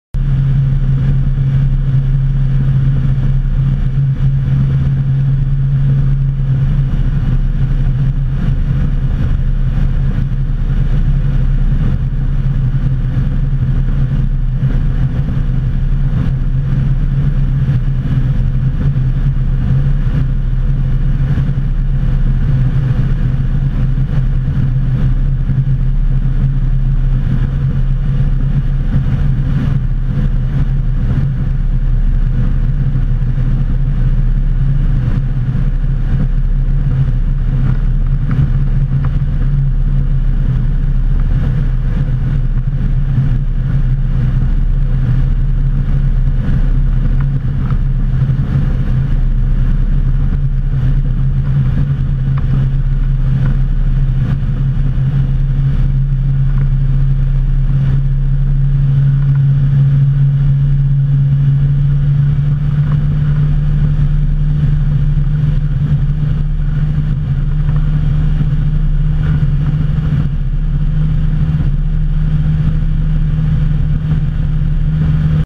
Lot nad Radomiem - Os. Gołębiów 2
Lot motolotnią nad Radomiem (Os. Gołębiów 2) z dnia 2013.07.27.